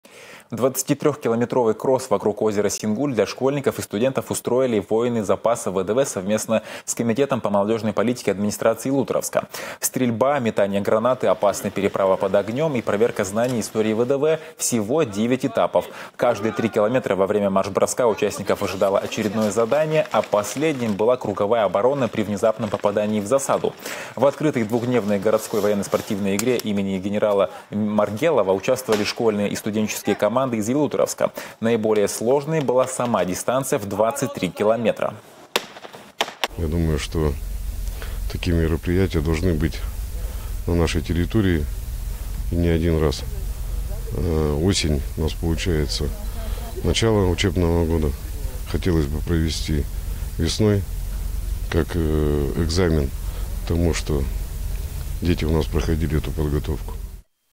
Марш - бросок
Ялуторовские десантники провели девятую военно-спортивную игру имени генерала Маргелова.